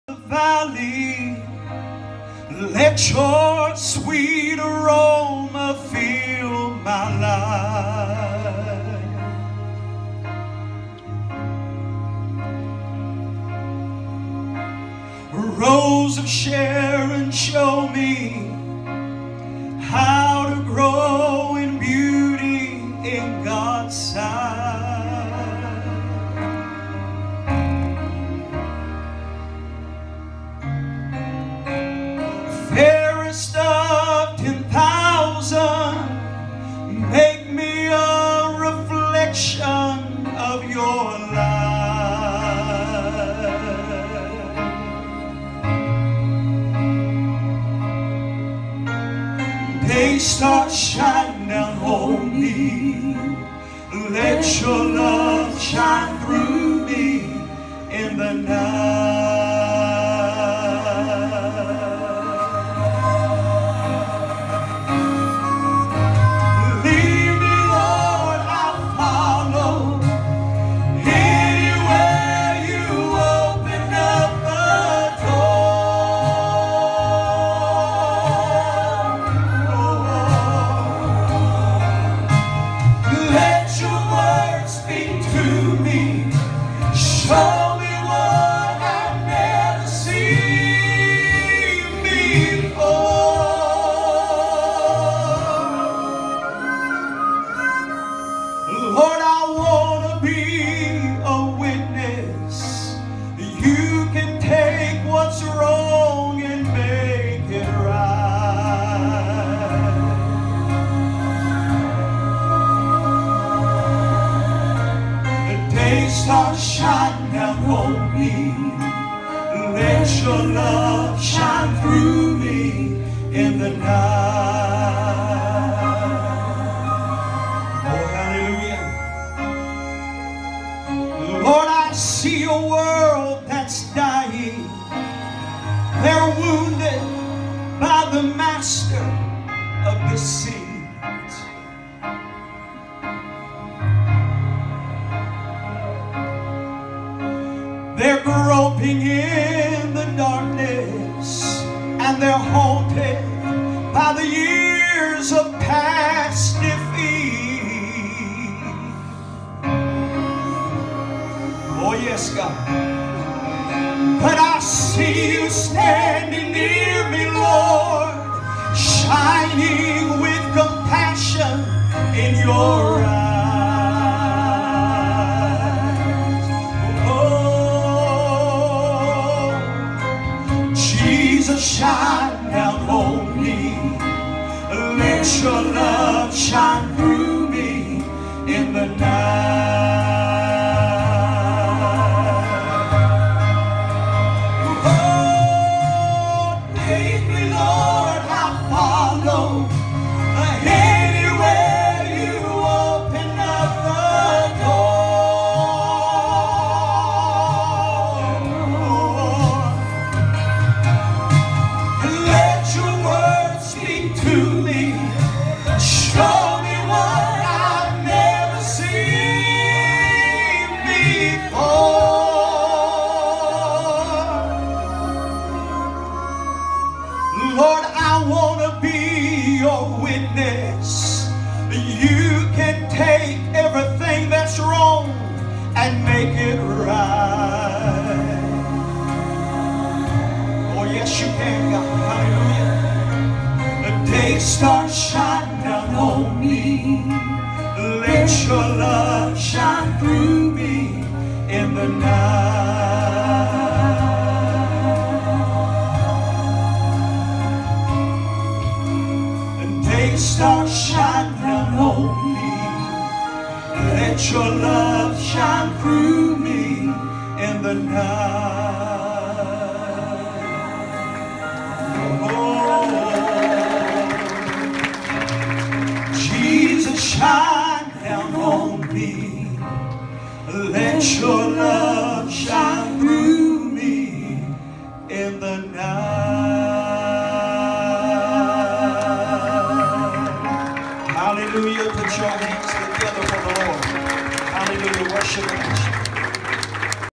Daystar by Pastors Caleb & Ashley Howard
Revival @ Living Sanctuary Church 3-13-16